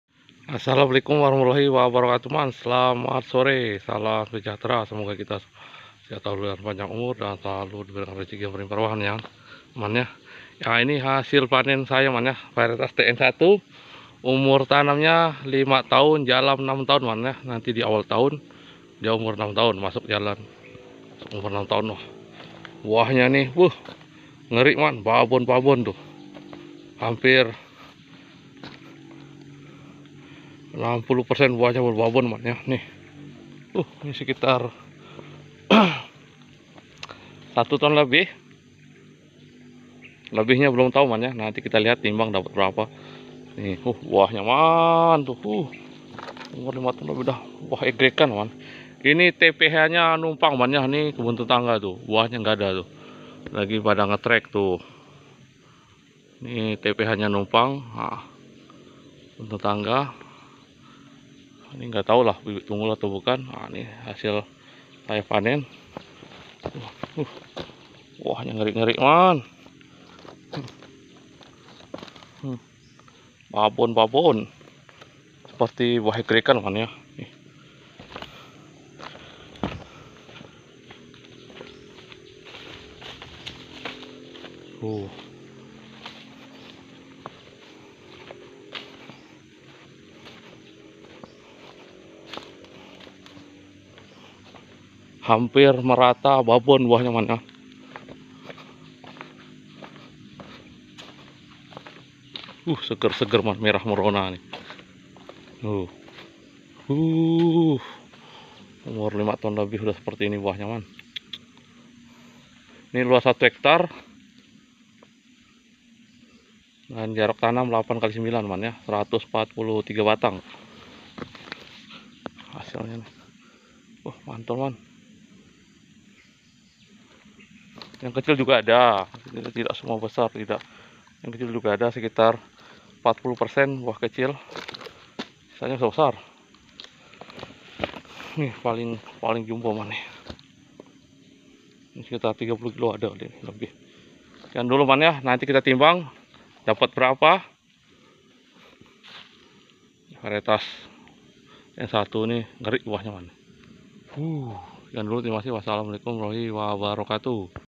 hasil panen sawit, TPH numpang kebun tetangga.dxp TN 1
Assalamualaikum teman2 salam sejahtera untuk kita semua di video kali ini saya akan menunjukkan hasil panen sawit unggul dxp tn 1 buah nya besar2